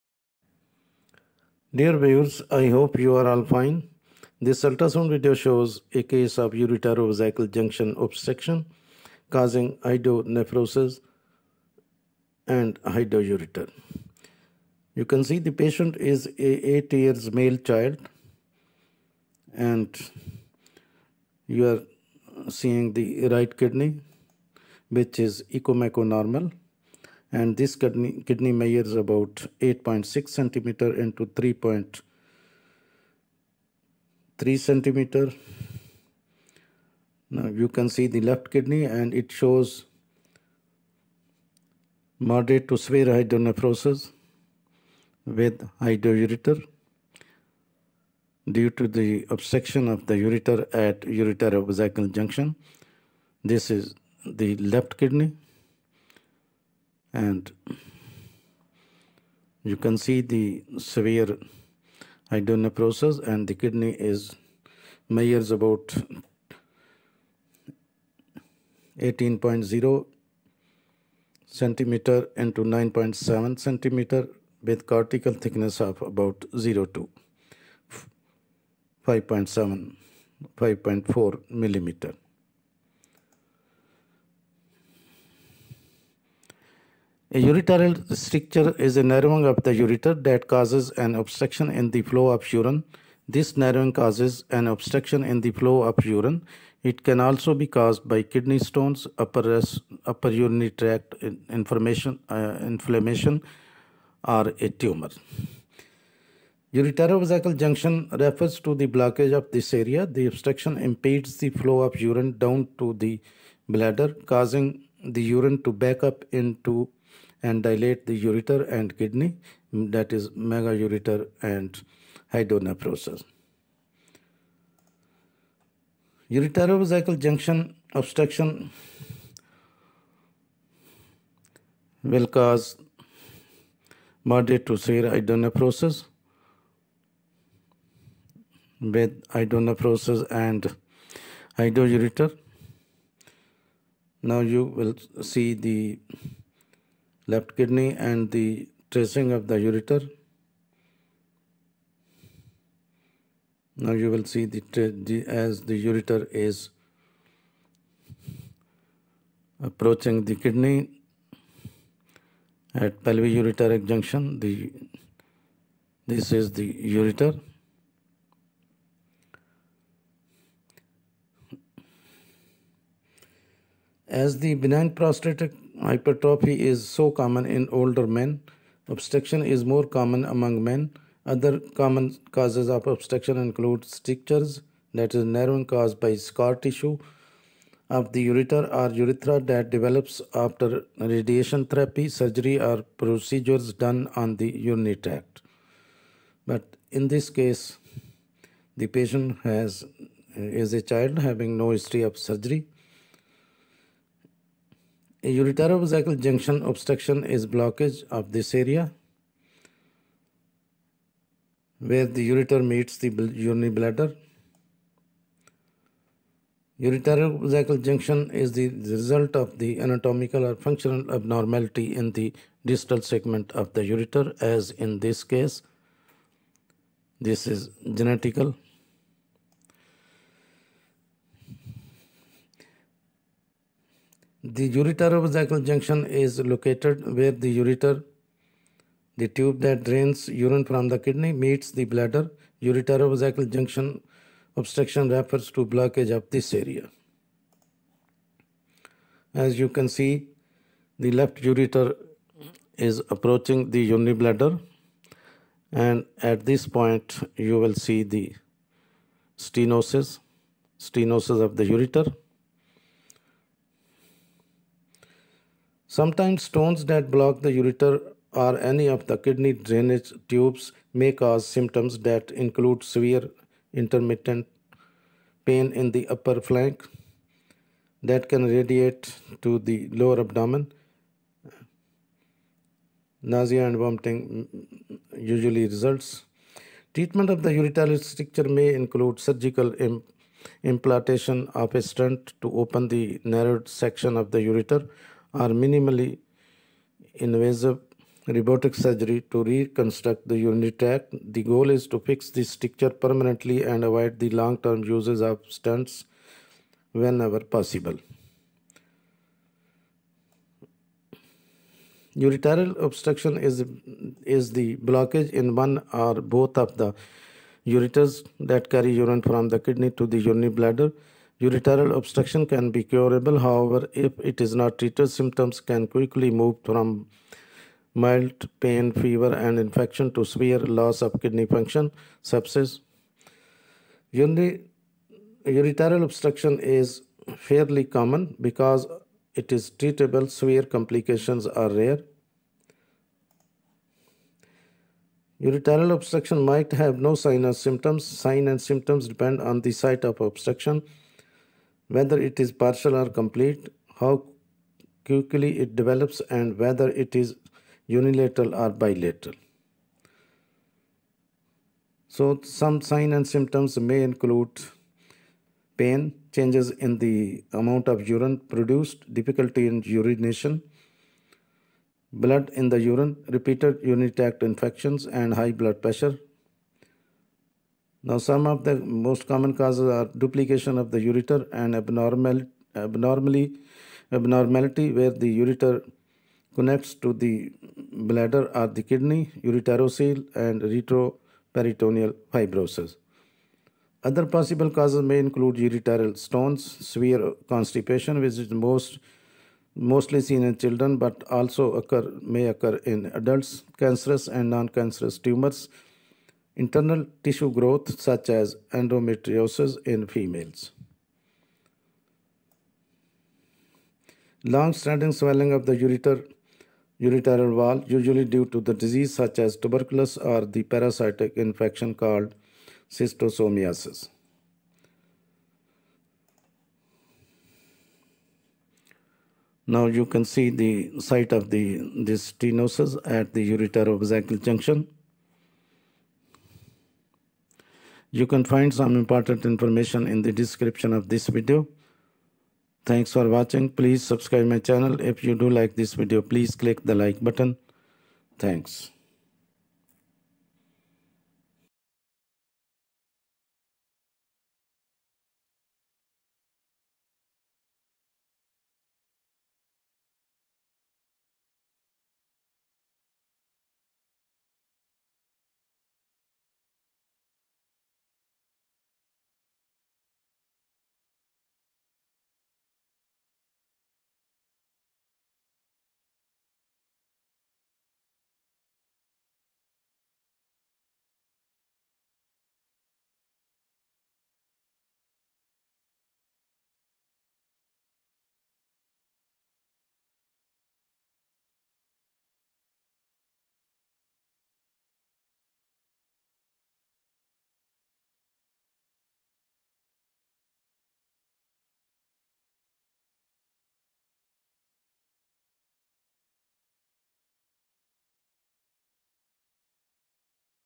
Ureterovesical junction (UVJ) obstruction, causing Hydronephrosis and Hydroureter.
This video shows Ureterovesical junction (UVJ) obstruction, causing Hydronephrosis and Hydroureter.
A ureteral stricture is a narrowing of the ureter that causes an obstruction in the flow of urine. This narrowing causes an obstruction in the flow of urine. It can also be caused by kidney stones, upper urinary tract inflammation, or a tumor.
Ureterovesical junction (UVJ) obstruction refers to a blockage to this area. The obstruction impedes the flow of urine down to the bladder, causing the urine to back up into and dilate the ureters and kidney (megaureter and hydronephrosis).
Because BPH is so common in older men, obstruction is more common among men. Other common causes of obstruction include strictures (narrowing caused by scar tissue) of the ureter or urethra that develop after radiation therapy, surgery, or procedures done on the urinary tract.
A ureterovesical junction (UVJ) obstruction is a blockage to the area where the ureter meets the bladder.
Ureterovesical junction obstruction (UVJO) is the result of an anatomic or functional abnormality in the distal segment of the ureter.
What is the ureterovesical junction (UVJ) obstruction? The ureterovesical junction is located where the ureter (the tube that drains urine from the kidney) meets the bladder. Ureterovesical junction (UVJ) obstruction refers to a blockage to this area.
Stones that block the ureter or any of the kidney's drainage tubes may cause symptoms that include: Severe, intermittent (comes and goes) pain in the upper flank (in the back, under the lower ribs) that can radiate (spread) to the lower abdomen, and; Nausea and vomiting.
Treatment for urethral stricture may include surgical implantation of a stent to open the narrowed section of the ureter or minimally invasive robotic surgery to reconstruct the urinary tract. The goal is to fix the stricture permanently and avoid the long-term use of stents, whenever possible.
Ureteral obstruction is a blockage in one or both of the tubes (ureters) that carry urine from your kidneys to your bladder. Ureteral obstruction can be curable. However, if it's not treated, symptoms can quickly move from mild — pain, fever, and infection — to severe — loss of kidney function, sepsis, and death.
Ureteral obstruction is fairly common. Because it's treatable, severe complications are rare.
Ureteral obstruction might have no signs or symptoms. Signs and symptoms depend on the site of obstruction, whether it's partial or complete, how quickly it develops, and whether it is unilateral or bilateral.
                         Signs and symptoms might include:-
Pain
Changes in the amount of urine produced
Difficulty in urinating
Blood in the urine
Repeated urinary tract infections
High blood pressure (hypertension)
                                                            Causes
Duplication of the ureter.
An abnormality where the ureter connects to the bladder or the kidney.
Ureterocele.
Retroperitoneal fibrosis.
                                                  Other possible causes
Ureteral stones
Severe constipation, which is mostly seen in children but also occurs in adults
Cancerous and noncancerous tumors
Internal tissue growth, such as endometriosis in females
Long-standing swelling of the ureter wall, usually due to diseases such as tuberculosis or a parasite infection called schistosomiasis.